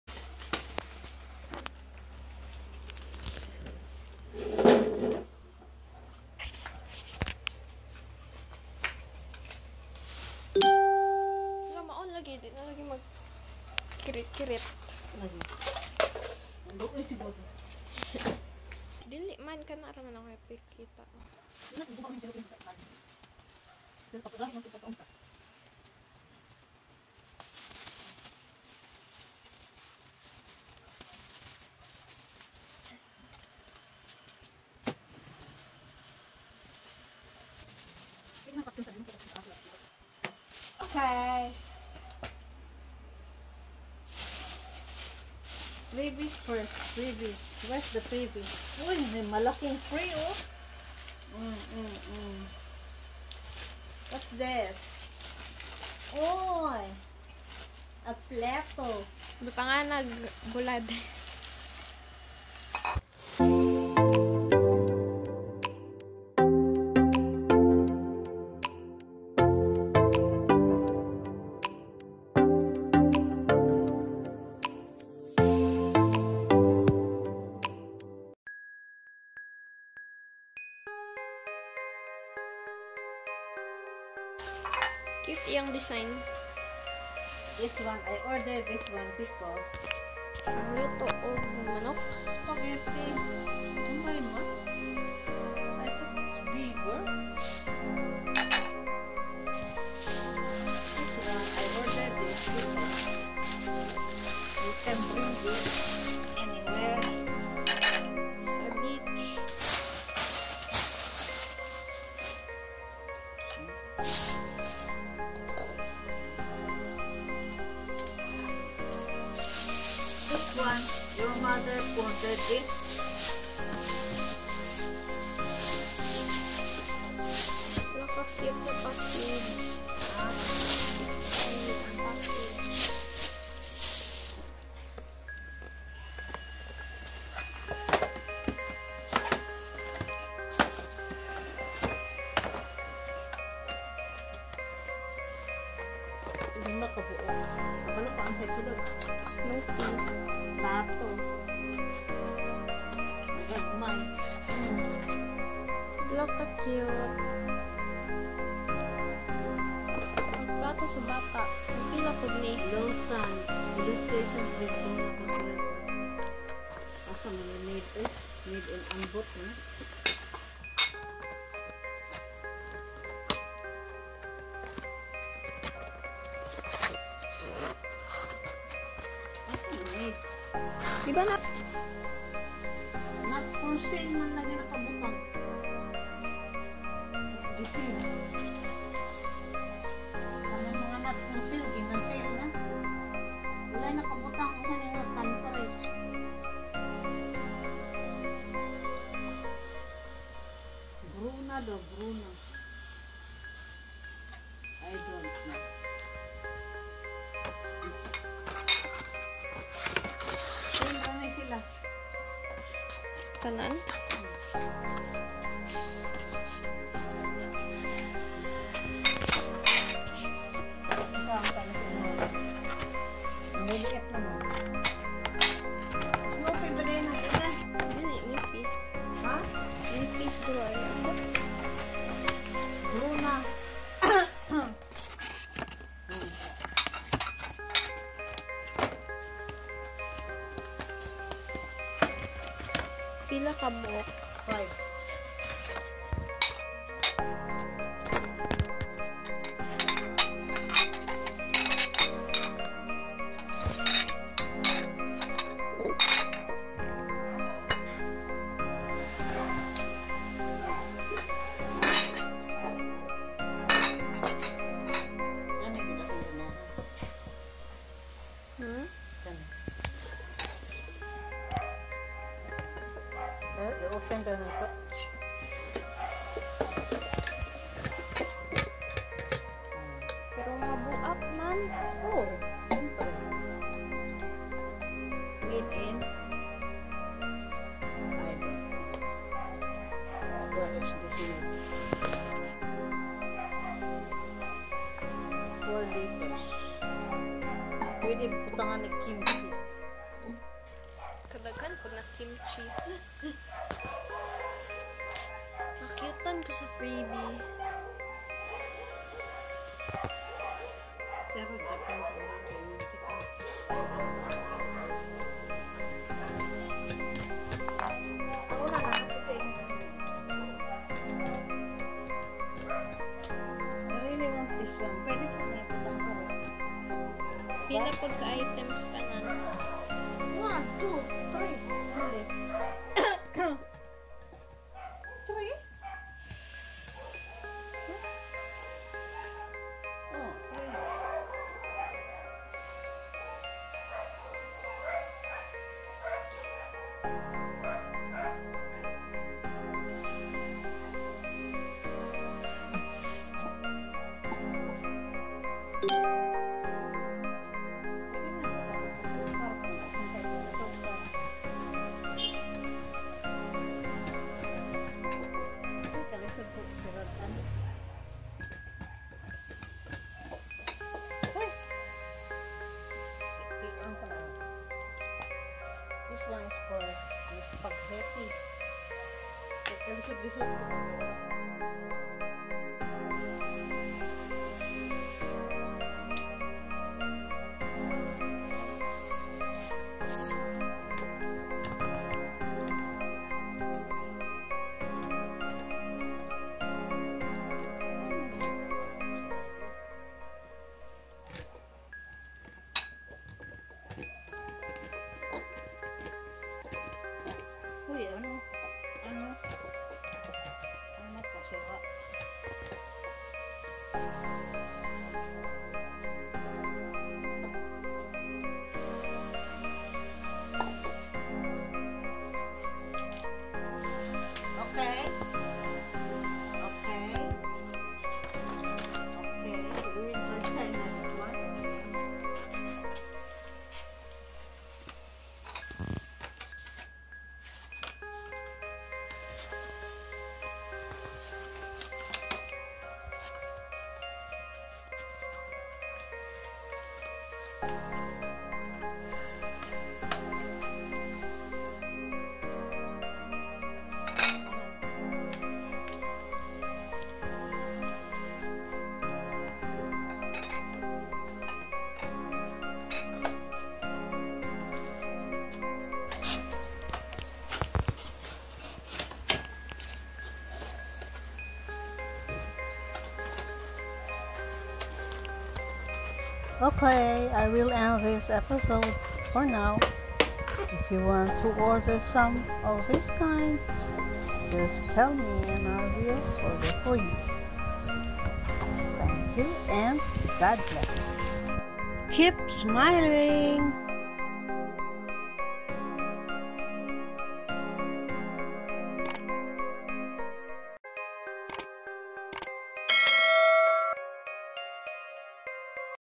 UNWRAPPING JAPAN SURPLUS KITCHEN WARES for reselling if somebody likes them | Rose Domingo |#222
2nd Delivery of japan surplus plates for the year for reselling purposes.
Thank you once again sa pagtangkilik sa small online business ko. God bless.🥰🥰🥰

Email add:
 rmignacio0507@yahoo.
   com

Gadgets:
 1.) Samsung galaxy
     S10+
 2.) IPAD 6th generation
 3.) Desktop Computer